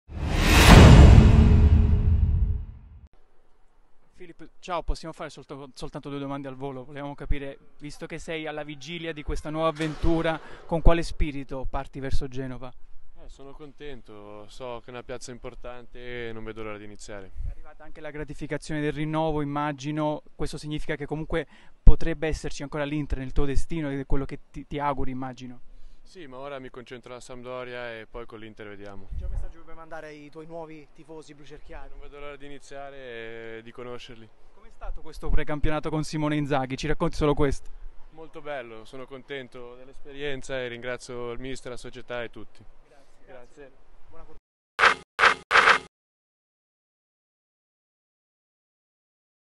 Stankovic rinnova con l'Inter e va alla Samp in prestito: 'Nerazzurri, poi ne riparliamo'
Un altro Stankovic alla Sampdoria dopo papà Dejan: Filip è pronto a difendere i pali che erano di Audero, passato in nerazzurro.